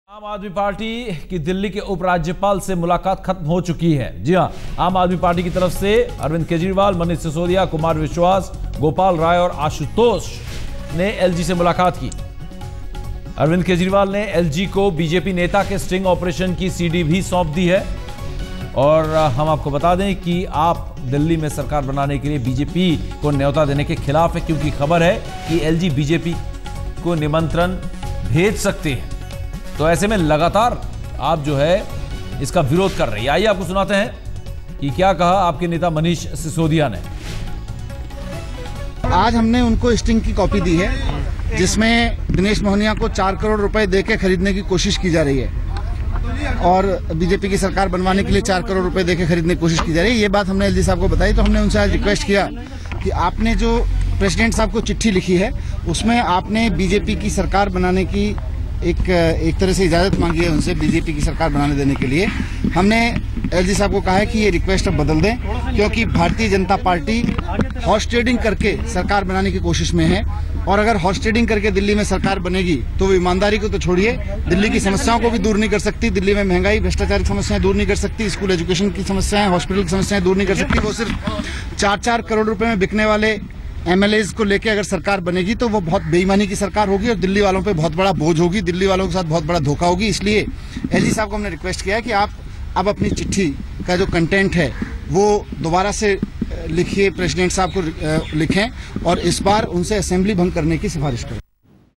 Arvind Kejriwal gives sting video to Delhi Lt-Gov
AAP chief Arvind Kejriwal has submitted a copy of the sting video, in which a BJP leader is seen poaching an AAP MLA, to Delhi Lt-Gov Najeeb Jung. Kejriwal was accompanied by party leaders Manish Sisodia, Ashutosh, Gopal Rai and Kumar Vishwas.